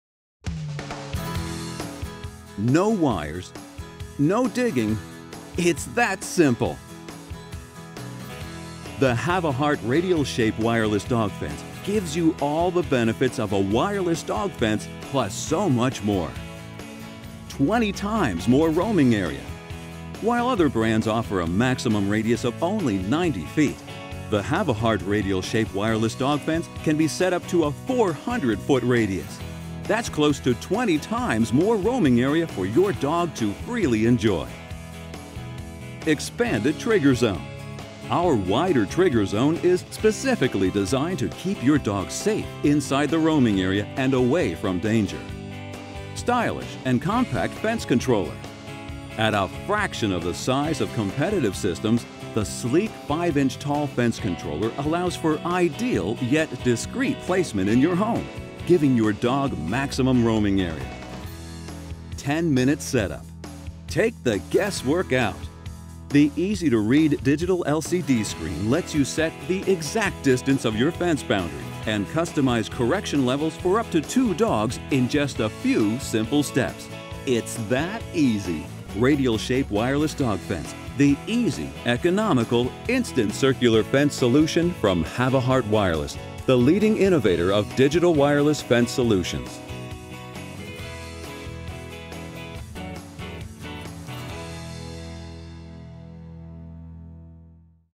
Radial-Shape Wireless Dog Fence with 400 Foot Roaming Area!
-- The Radial-Shape Havahart® Wireless Dog Fence is an easy-to-use, economical and efficient circular wireless dog containment solution.  This wireless fence will keep your dog safe while providing nearly 20 times more Roaming Area than other brands!

In this video, we explain how the Radial-Shape Havahart® Wireless Dog Fence will give you and your dog all the benefits of traditional wireless dog fences, plus so much more!

As the leading innovator in digital wireless and electric dog fences, the Radial-Shape Havahart® Wireless Dog Fence is easy to set up, and the easy-to-read digital LCD screen lets you set the exact size of your Roaming Area. Unlike radial wireless fences from other brands, the Radial-Shape Havahart® Wireless Dog Fence has a maximum 400 foot radius, which provides your dog with significantly more room to exercise and explore!

With the Radial-Shape Havahart® Wireless Dog Fence, you are able to set the correction level to one that is appropriate for your dog's temperament, age and size. In addition, this system features a 6 to 13 foot Trigger Zone, which keeps your dog safe. This wider Trigger Zone helps to deter your dog from leaving the Fence Boundary, since it gives him more time to be issued a correction and make the decision to turn back into the safety of the Roaming Area. Lastly, the Radial-Shape system includes a 30-second safety time-out feature, and it will not correct your dog when he returns to the Roaming Area.

To learn more about the Radial-Shape Havahart® Wireless Dog Fence and other wireless dog containment systems, please